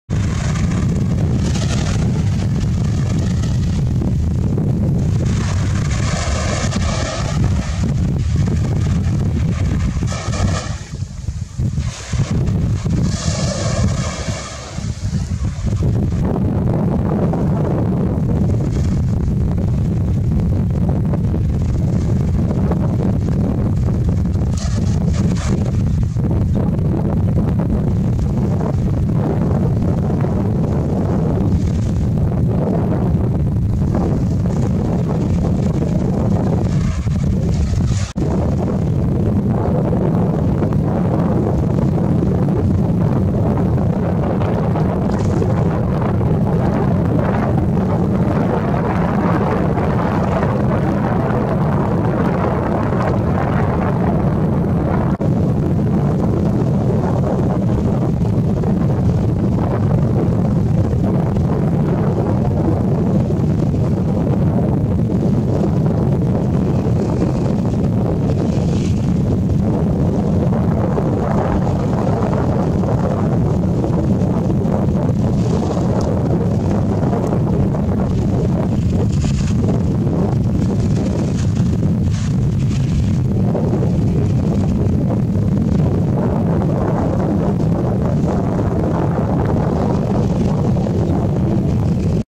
トウネン 秋の浜辺にて
2023年９月。石狩市。浜辺を歩いていて会えたトウネンの様子を動画撮影しました。